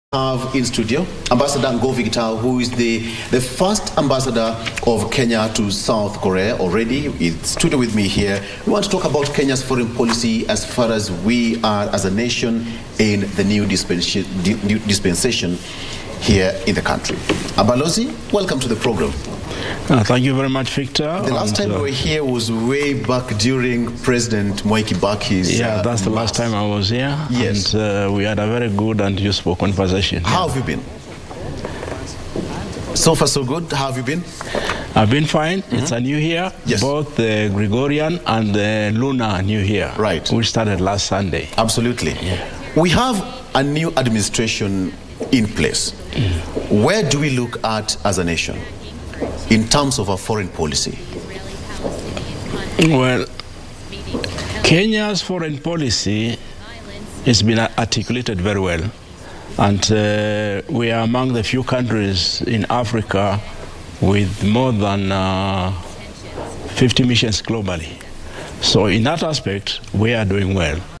Ngovi Kitau on Kenya's Post-Election Foreign Relations
Ngovi Kitau, Former Ambassador of Kenya to South Korea is in studio to talk about Kenya's Post-Election Foreign Relations.